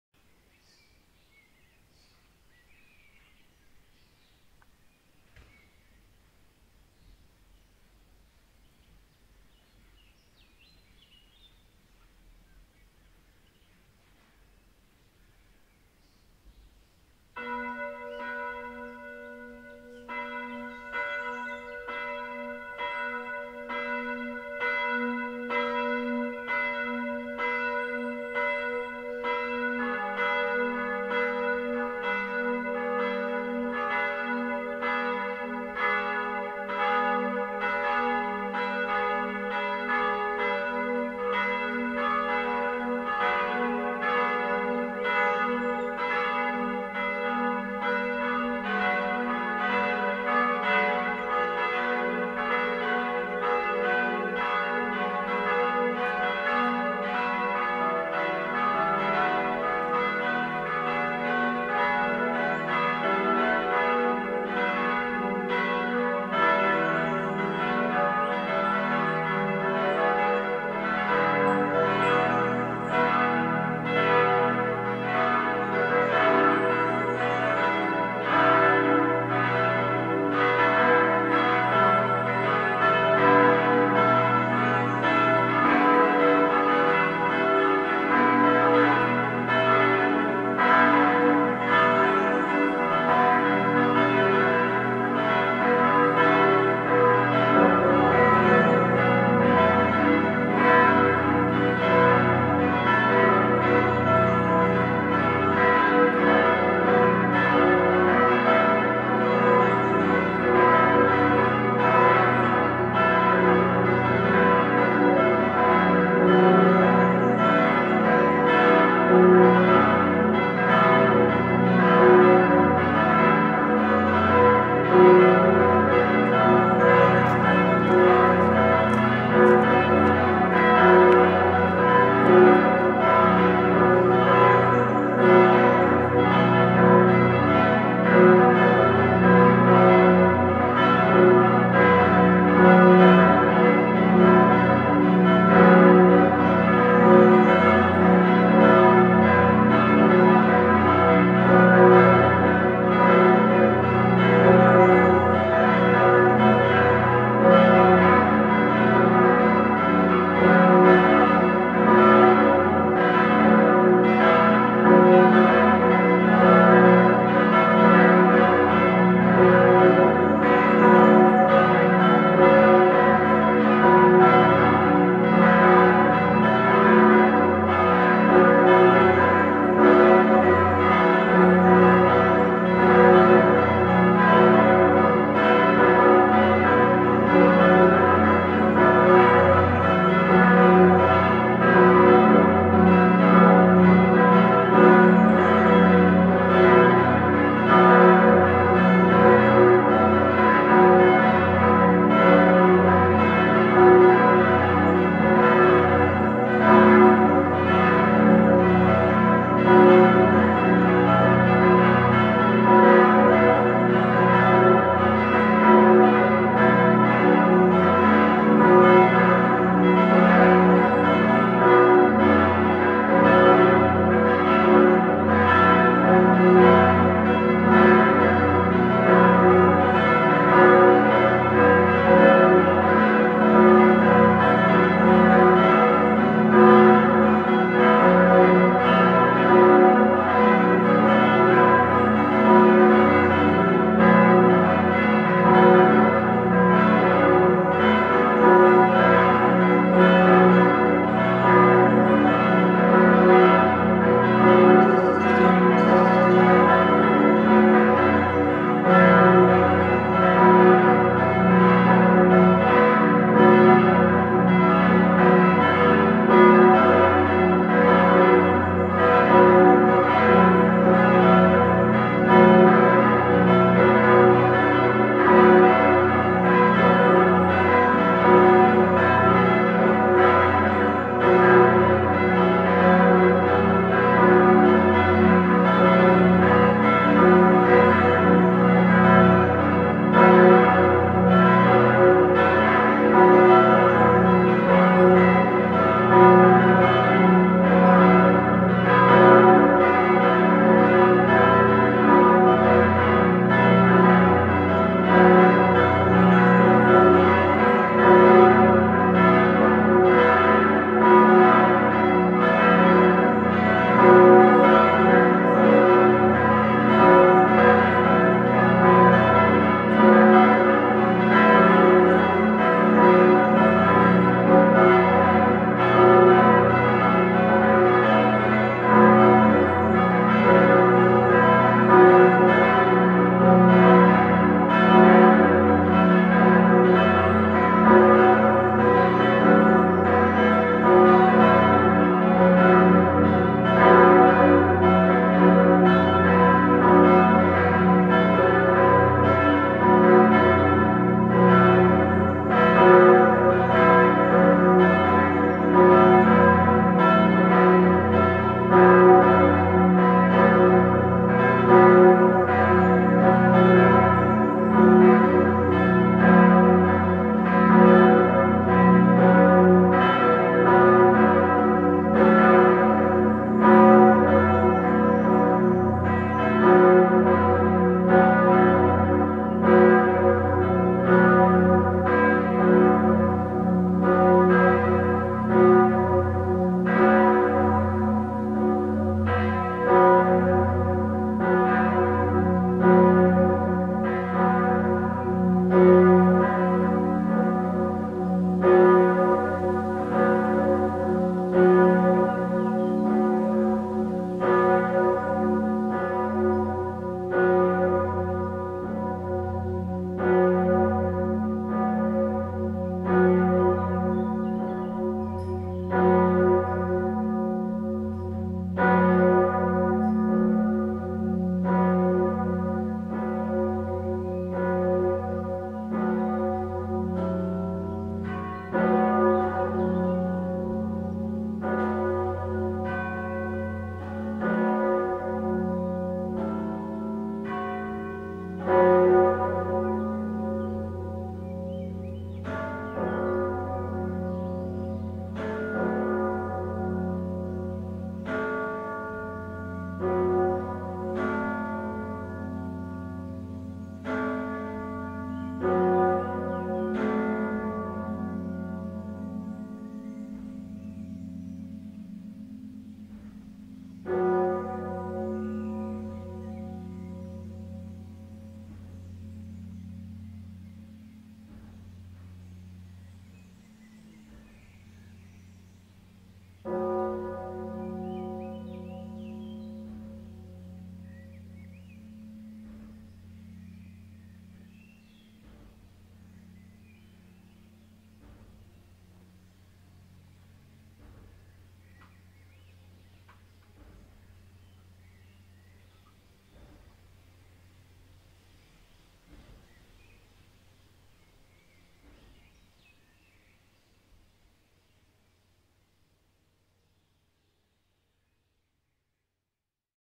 St. Ottilien LL Erzabteikirche zum Heiligsten Herzen Jesu Glocken 9 7 6 5 3 1
Disposition der hier zu hörenden Glocken: fis° h° e´ fis´ gis´ h´

Aufnahme am Donnerstag, den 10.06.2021 um 17:53 zur 1. Pontifikalvesper um 18:00 zum Patrozinium zum Herz Jesu-Fest.